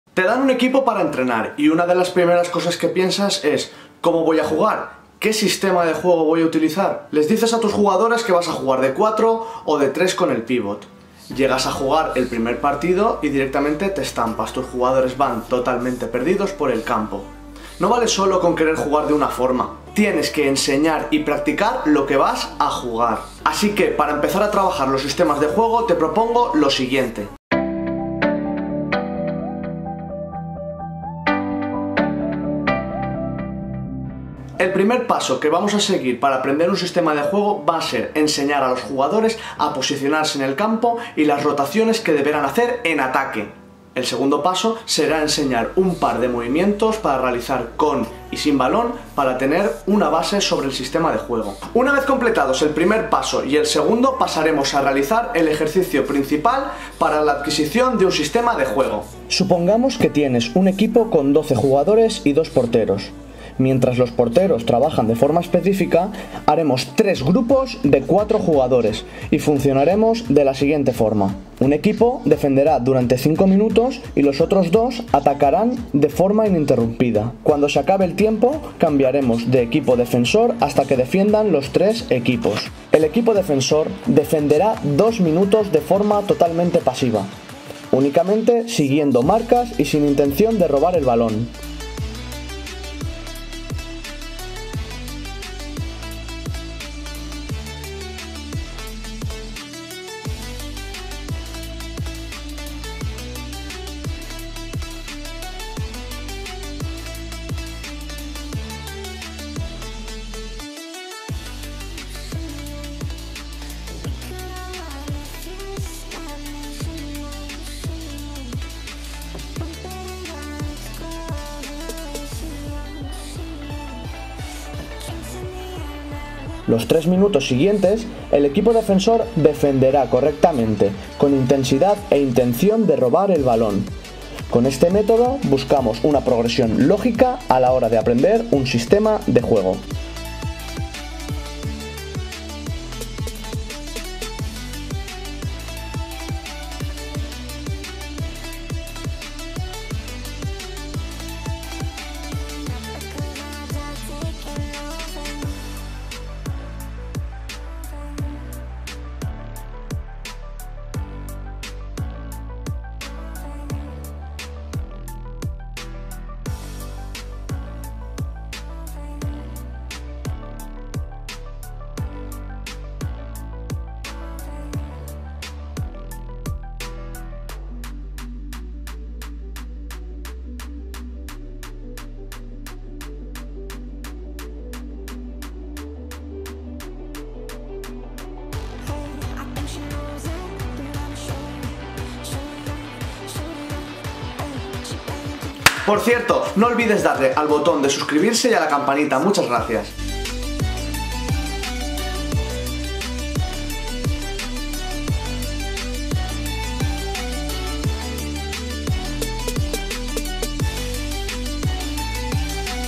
SISTEMAS de JUEGO en el FÚTBOL SALA - Cómo trabajarlos (4-0) (3-1)
¿Como trabajar 4-0 en fútbol sala? ¿Cómo trabajar 3-1 en futsal? Si quieres ver una explicación lógica de como trabajar los sistemas de juego en el futsal. ¡QUÉDATE A VERLO!